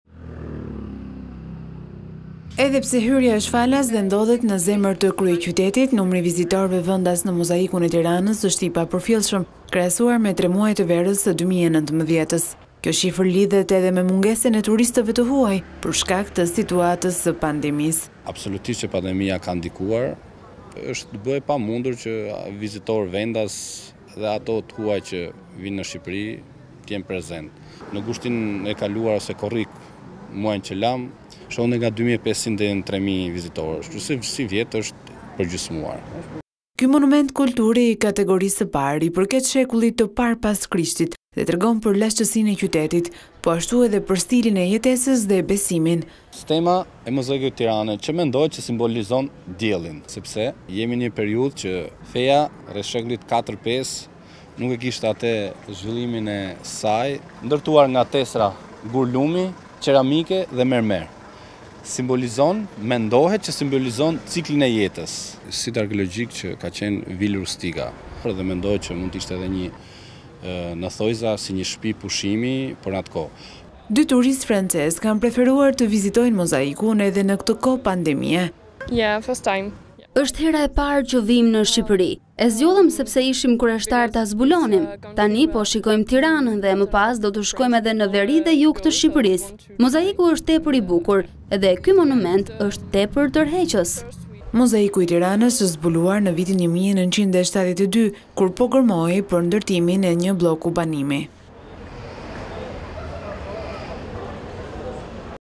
Pak turiste te mozaiku i Tiranes - News, Lajme - Vizion Plus
Edhe pse hyrja është falas, dhe ndodhet në zemër të kryeqytetit numri i vizitorëve vendas në Mozaikun e Tiranës është i papërfillshëm, krahasuar me tre muajt e verës së 2019. Kjo shifër lidhet edhe me mungesën e turistëve të huaj për shkak të situatës së pandemisë. LUAN NEZHA ARKEOLOG Ky monument kulture i kategorisë së parë i përket shekullit të parë pas Krishtit dhe tregon për lashtësinë e qytetit, po ashtu edhe për stilin e jetesës dhe besimin. LUAN NEZHA ARKEOLOG Dy turistë francezë kanë preferuar të vizitojnë Mozaikun edhe në këtë kohë pandemie.TURISTMozaiku i Tiranës është zbuluara në vitin 1972, kur po gërmohej për ndërtimin e një blloku banimi.